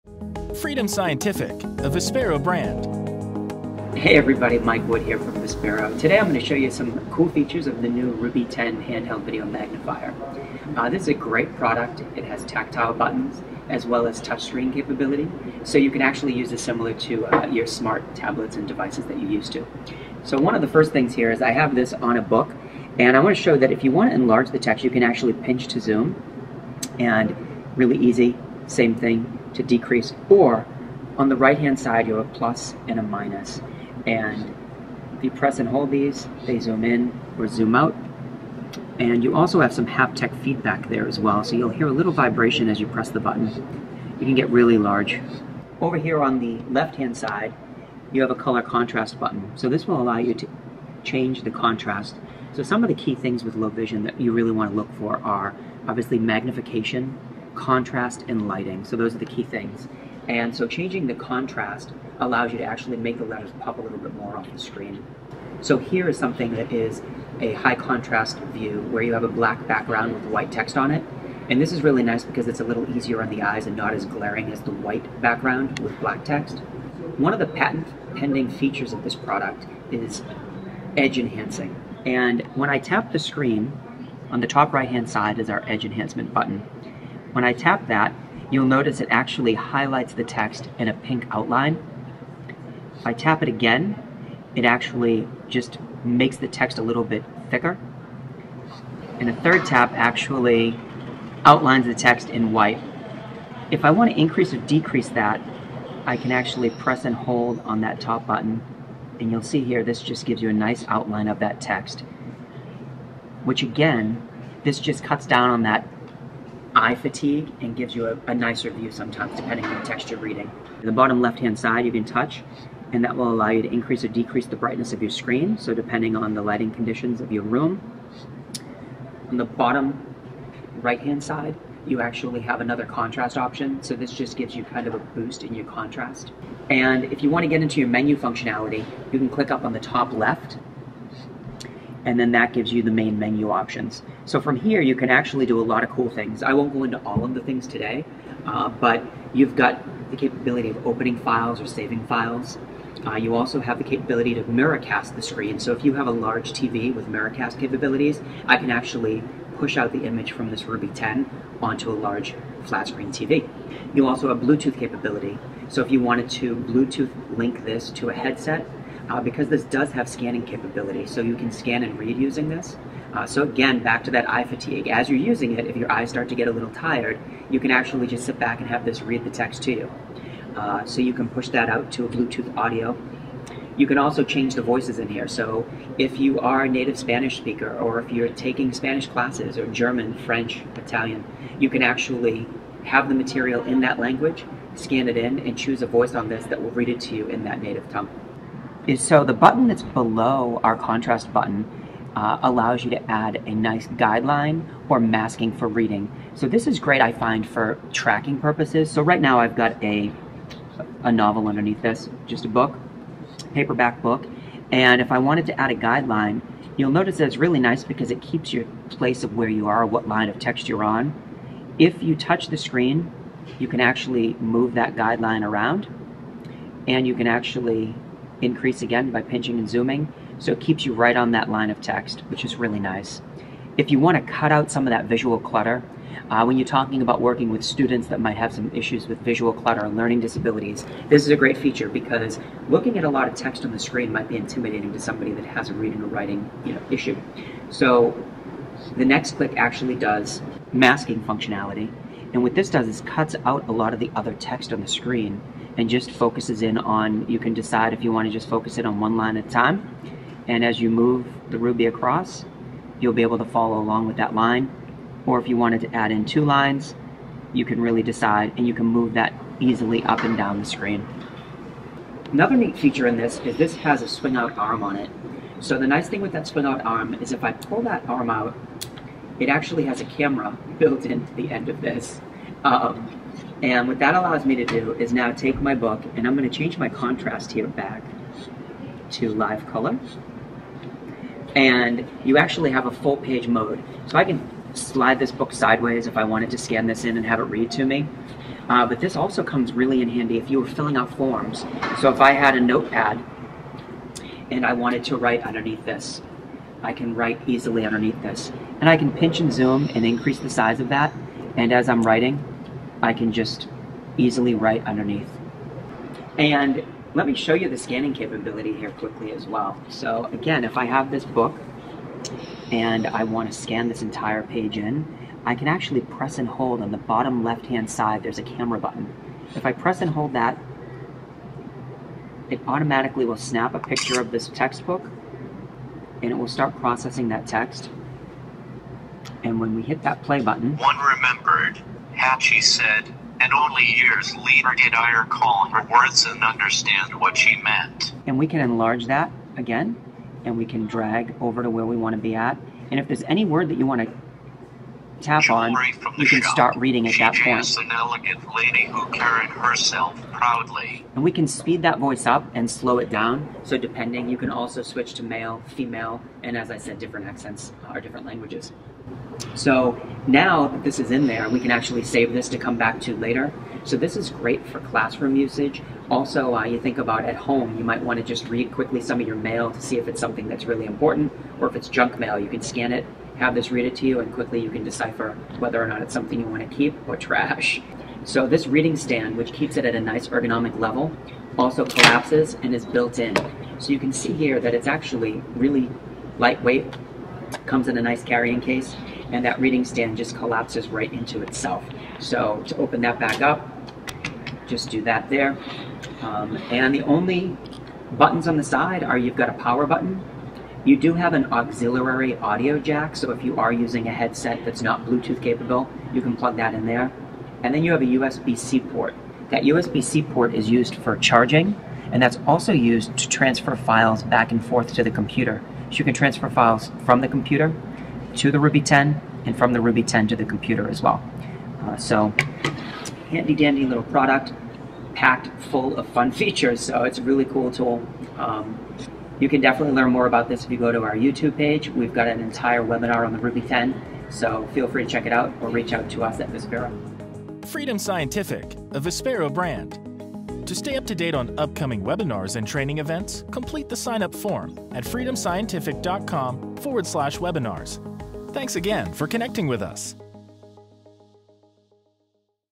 RUBY 10 Demonstration of Features
In this informative video, Mike Wood demonstrates some of the various features of the RUBY 10.